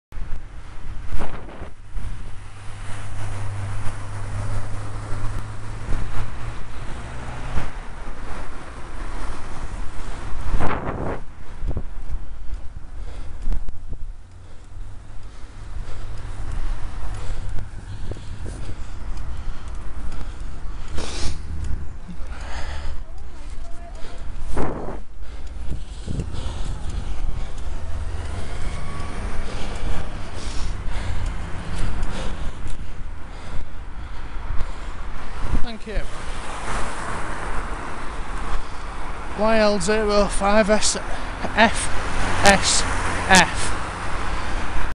YL05FSF - Patient, wide berth pass
Not all bad today, a thank you to this driver for waiting through the pinch points.